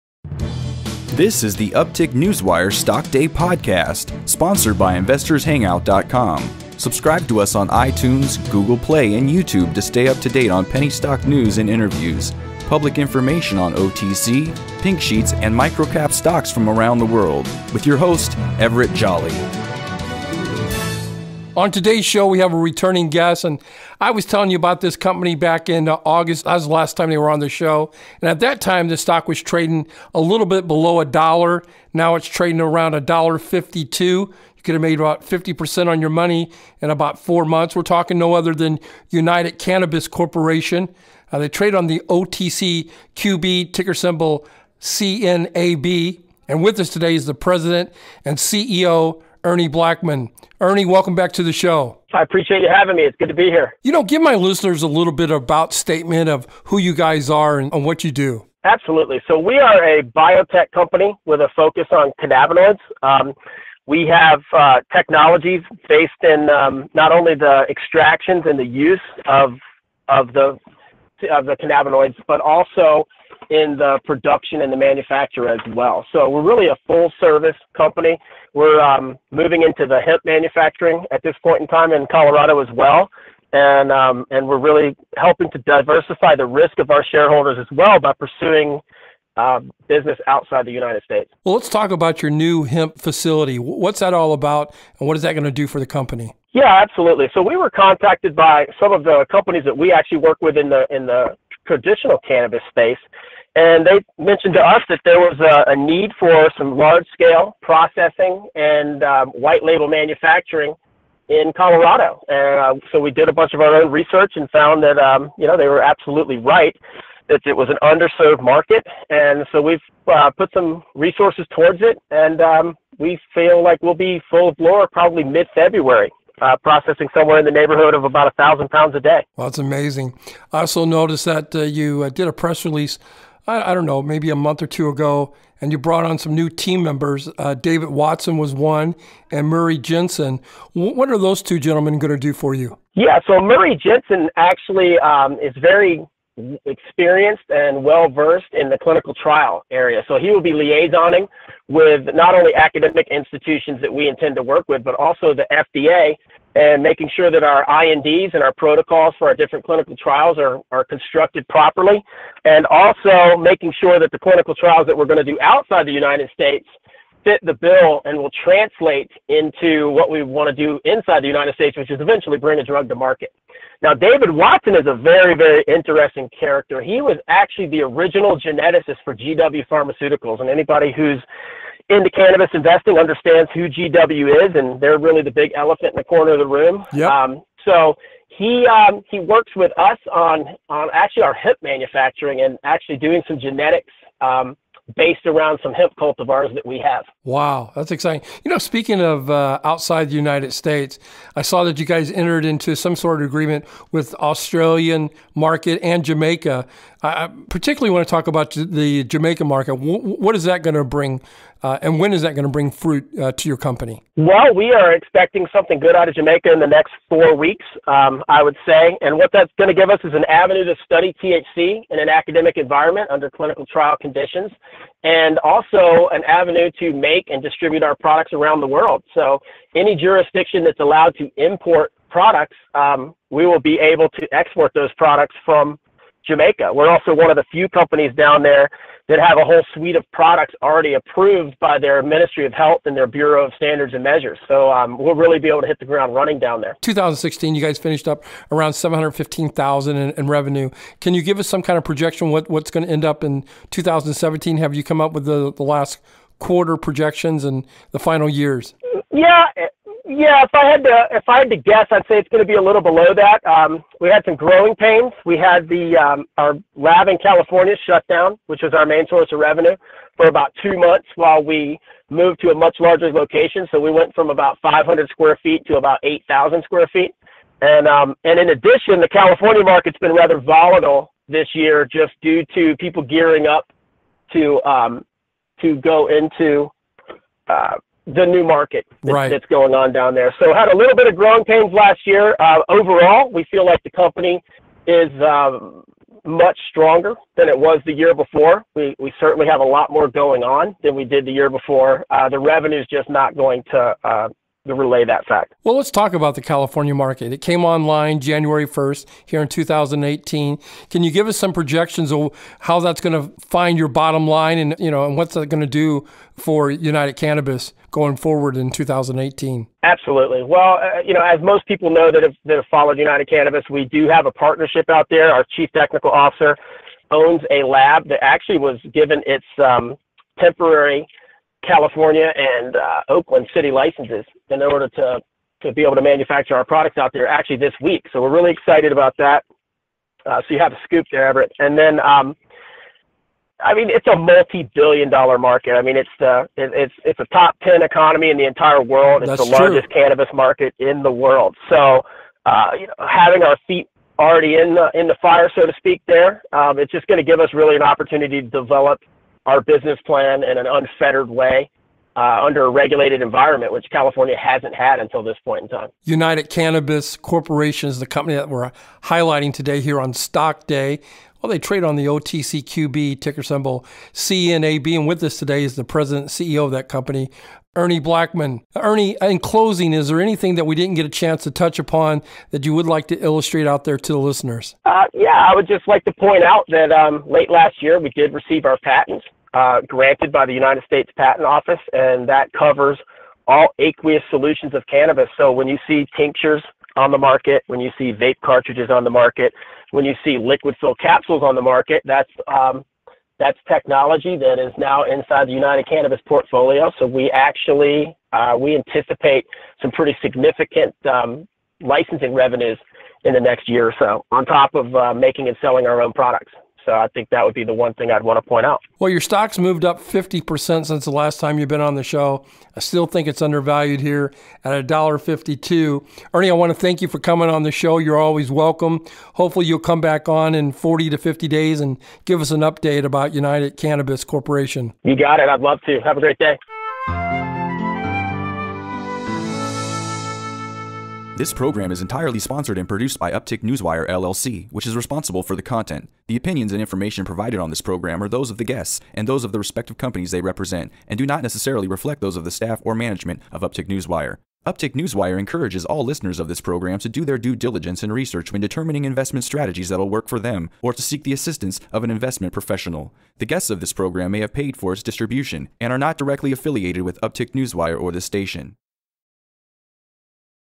CEO Earnie Blackmon of United Cannabis Corp. (OTCQB: CNAB) - Jan '18 update
CEO Earnie Blackmon of United Cannabis Corp. (OTCQB: CNAB), returns to Stock Day to give details on biotech, cannabinoids, and the opening of their hemp processing facility in Colorado.